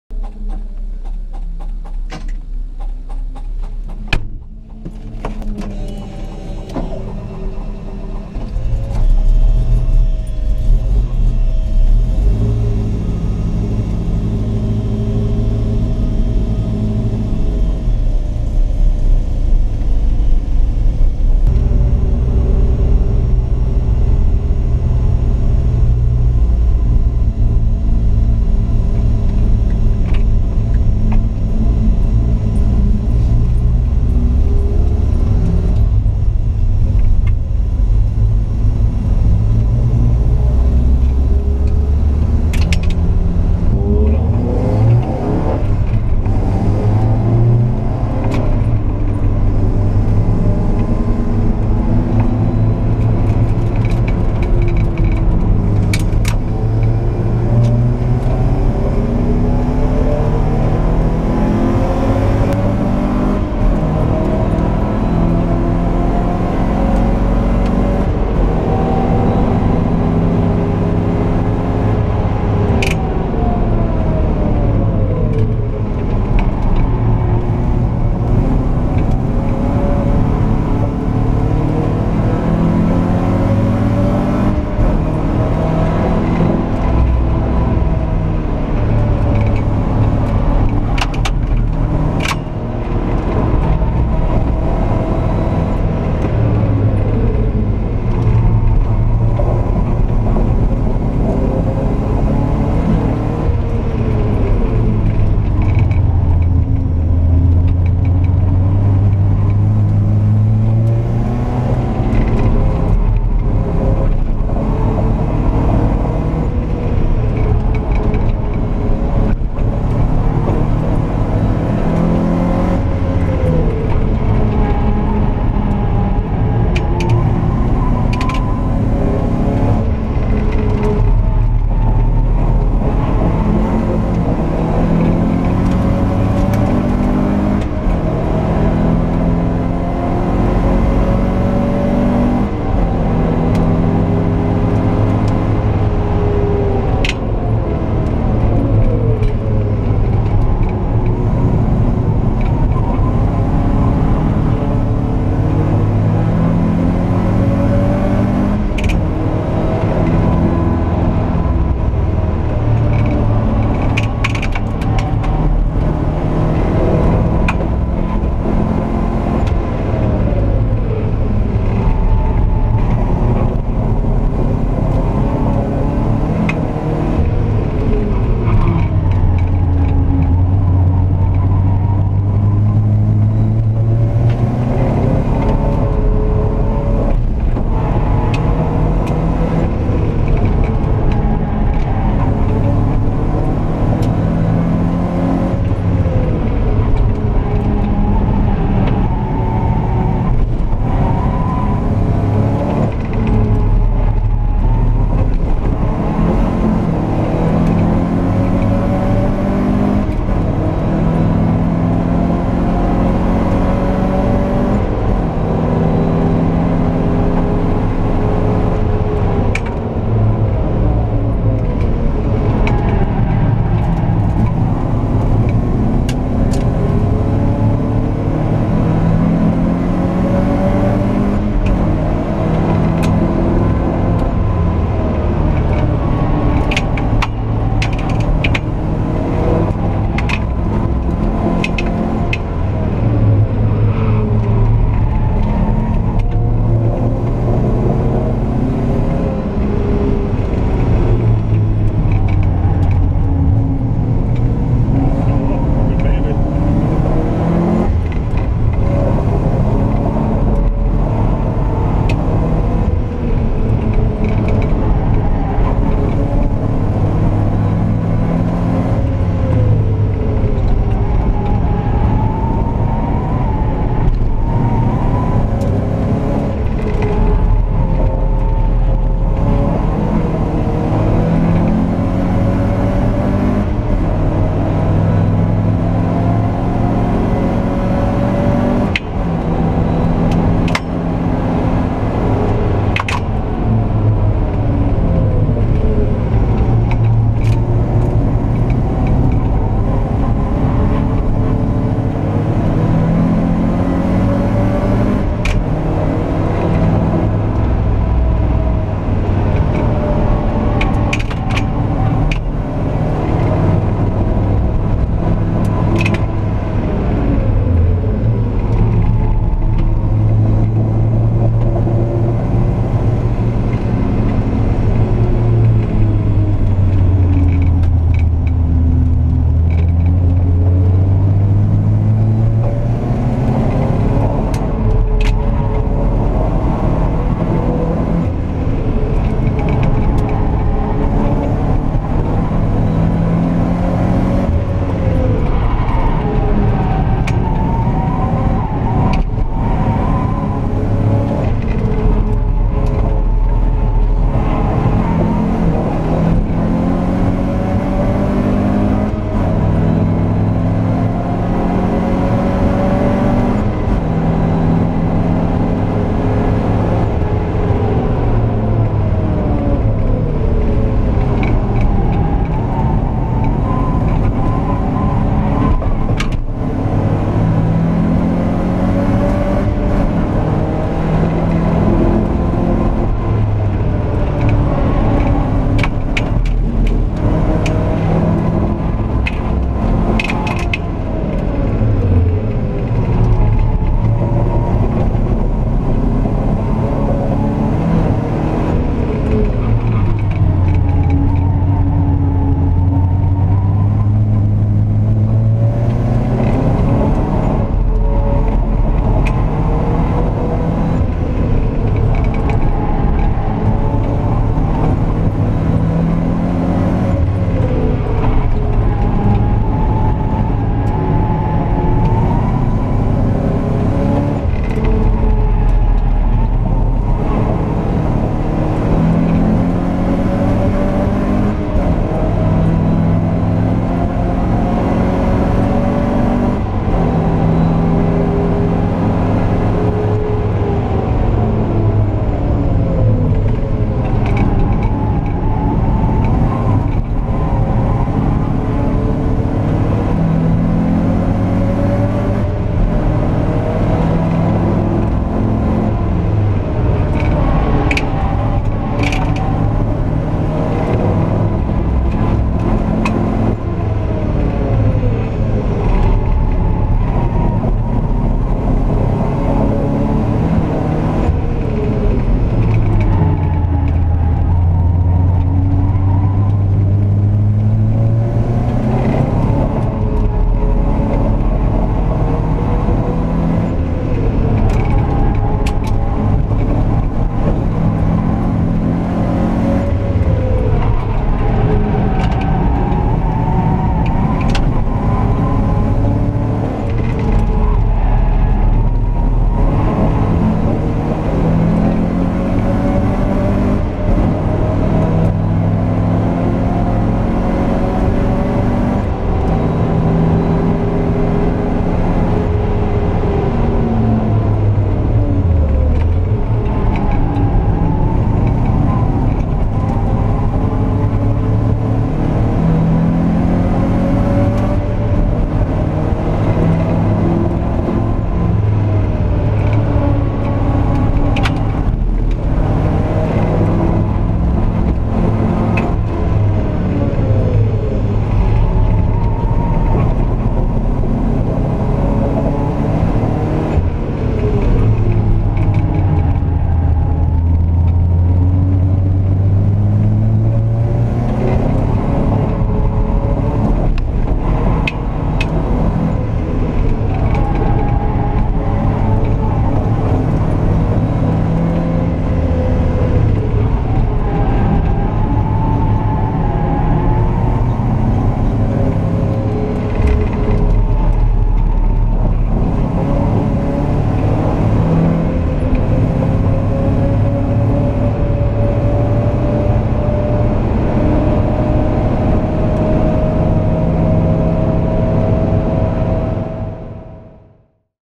Alfa Romeo Alfasud 1.5 TI chasing down E46 M3 on Ring Knutstorp
Peter Dyrelund is chasing down a track prepared BMW E46 M3 on Ring Knutstorp before having a bit of fun with a Lotus Elise.